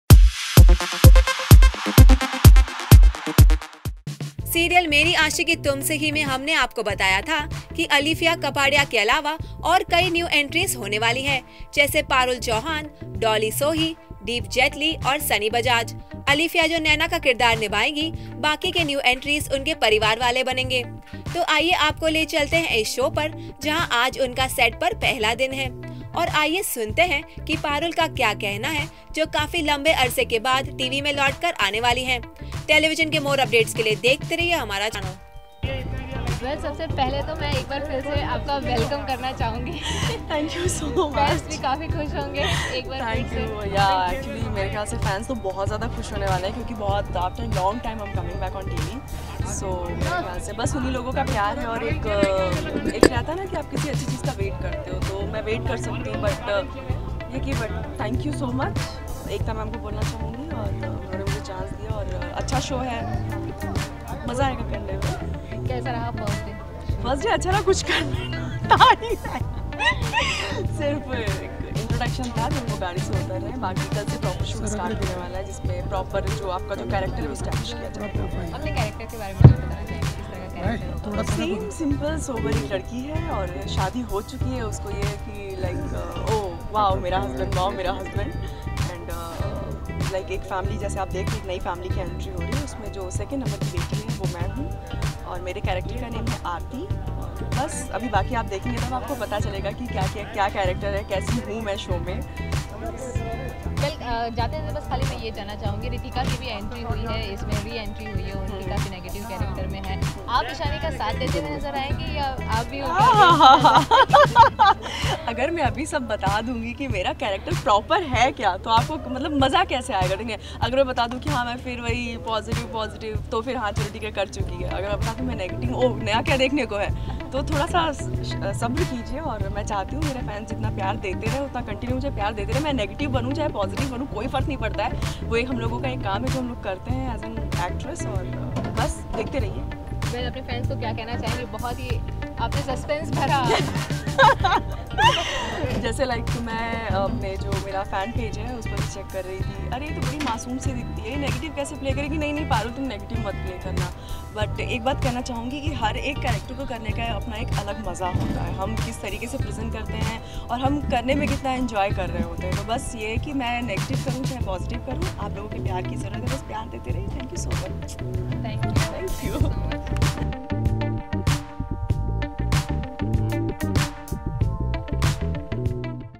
Meri Aashiqui Tumse Hi | New Entries First Day On The Show | 17th Dec 2015
Serial Meri Aashiqui Tumse Hi mei humne aapko bataya tha, ki Alefia Kapadia ke alawa aur kayi new entries hone waali hai, jaise Parul Chauhan, Dolly Sohi, Deep Jaitley aur Sunny Bajaj. Alefia jo Naina ka kirdhar nibhaegi, baaki ke new entries unke parivaar waale banege. Toh aaiye aapko le chalte hai is show par, jahan aaj unka set par pehla din hai. Aur aayiye sunte hai ki Parul ka kya kehna hai jo kaafi lambe arse ke baad TV mei lautkar aane wali hai. Telivision ke more updates ke liye dekhte rahiye humara channel.